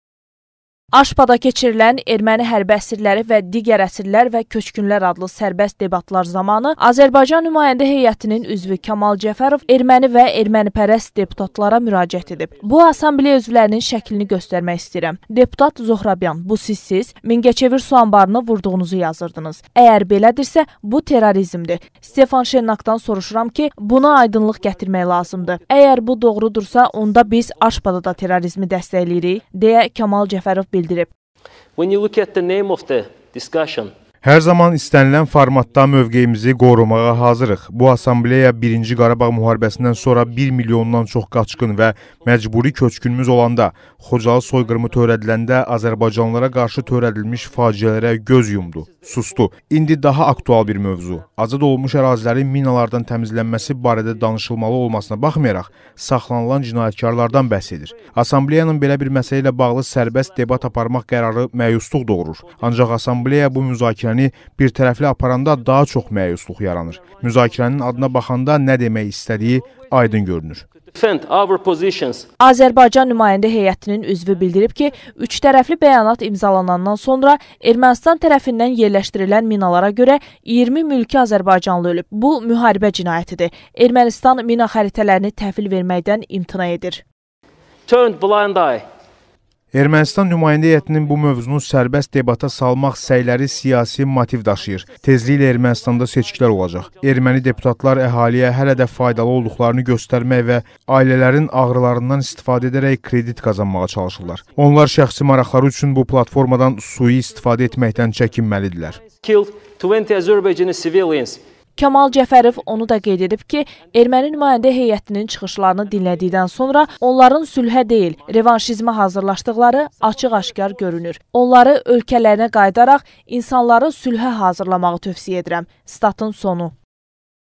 AŞPA da ermənilərə cavab
Kamal Cəfərov: Erməni nümayəndə heyəti sülhə deyil, revanşizmə hazırlaşır